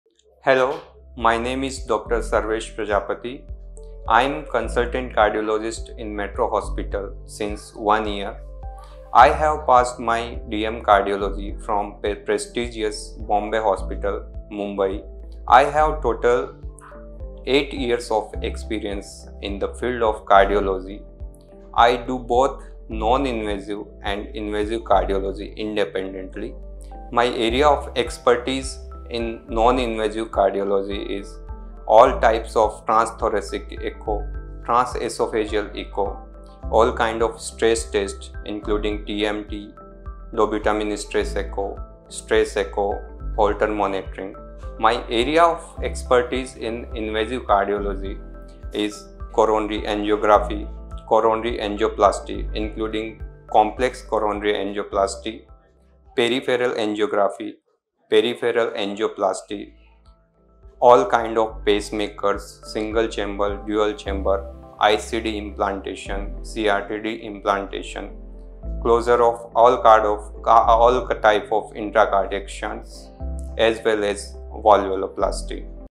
Dr. Sravesh Kumar Prajapati: Cardiology Consultant with 7+ Years in Heart Care & Advanced Procedures
Meet Dr. Sravesh Kumar Prajapati, a highly experienced cardiology consultant with over 7 years of expertise. Specializing in non-invasive cardiology, transesophageal echocardiography, EMT procedures, invasive coronary angiography, coronary angioplasty, and pacemaker implantation, Dr. Prajapati is dedicated to delivering exceptional heart care and improving patient outcomes.

Available for consultations:
Monday to Saturday,
10:00 AM – 4:00 PM.